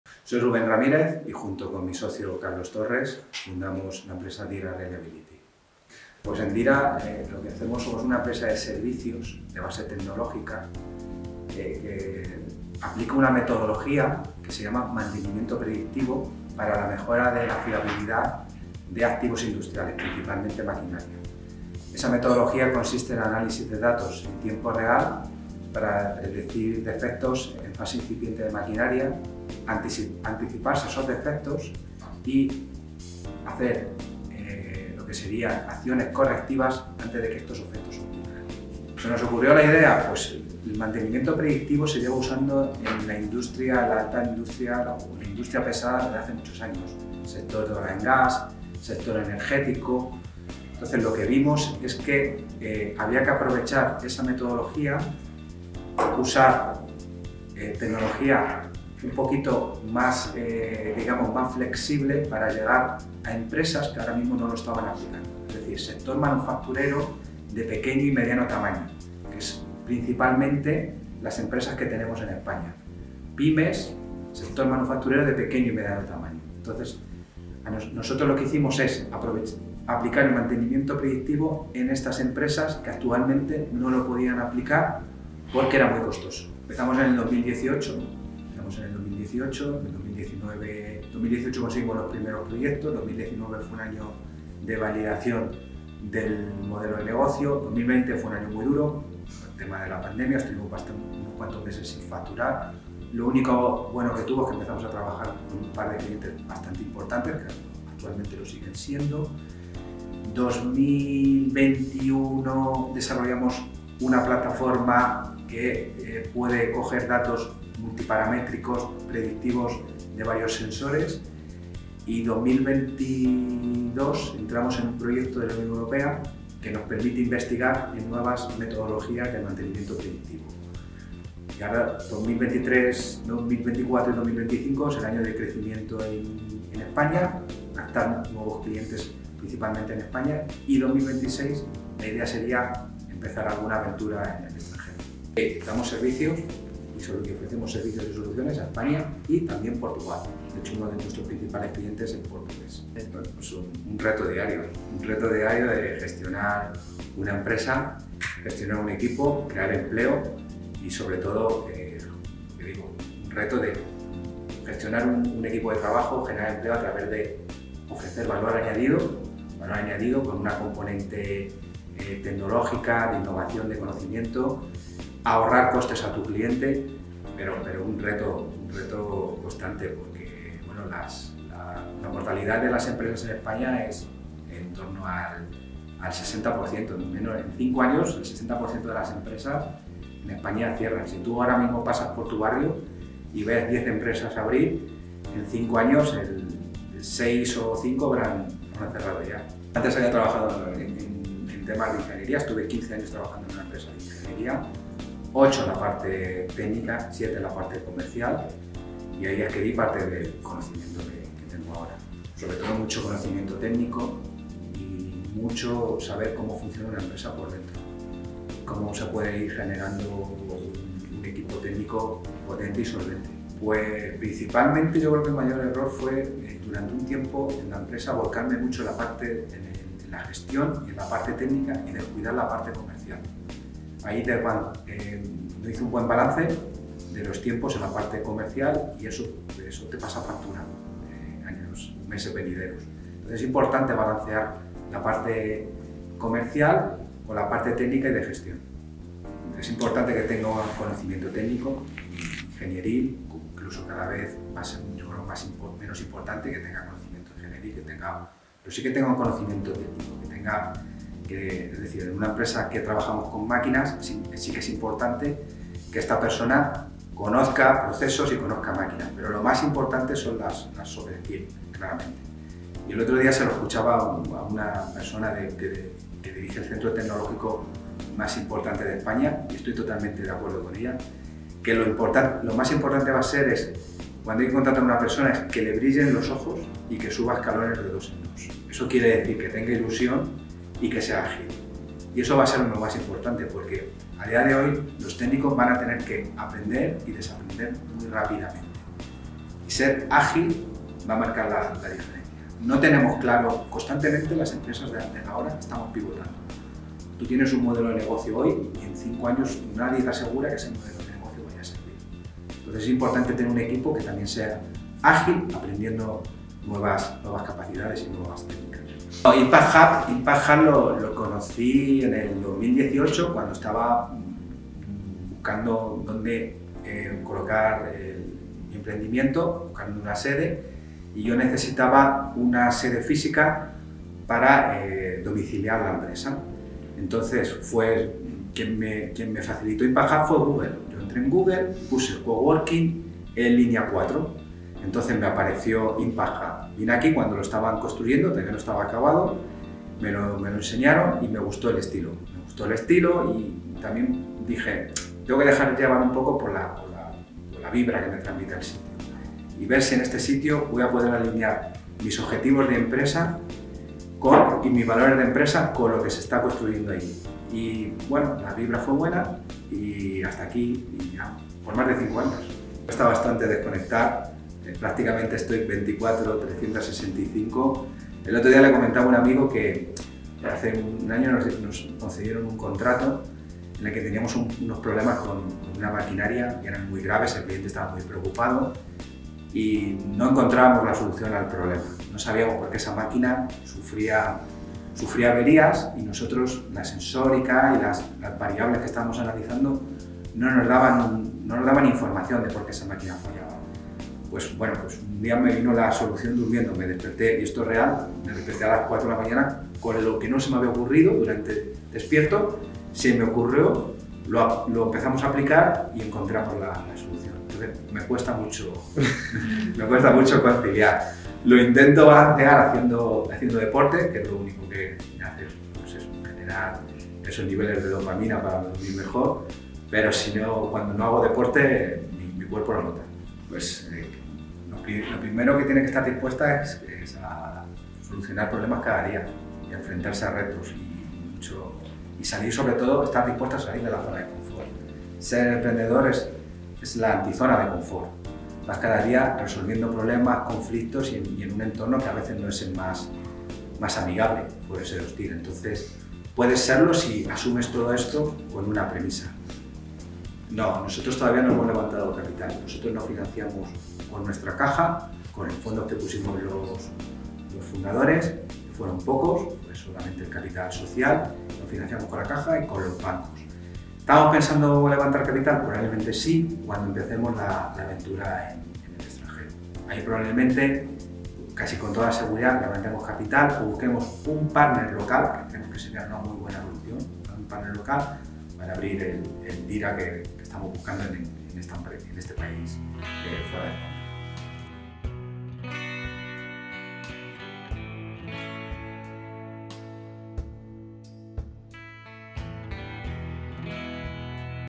Rubén Ramírez y Carlos Torres: Dirá Reliability
Rubén Ramírez y su socio Carlos Torres fundaron la empresa Dira Reliability con un enfoque innovador.

 Aunque el mantenimiento predictivo se ha utilizado en la industria pesada durante muchos años, en sectores como el gas y la energía, vieron la oportunidad de aprovechar esta metodología con tecnología más flexible. Su objetivo era llegar a pequeñas y medianas empresas del sector manufacturero en España, que anteriormente no podían aplicar estas técnicas debido a su alto costo.

Con Dira, han logrado hacer accesible el mantenimiento predictivo para estas PYMEs, mejorando la fiabilidad de sus activos industriales.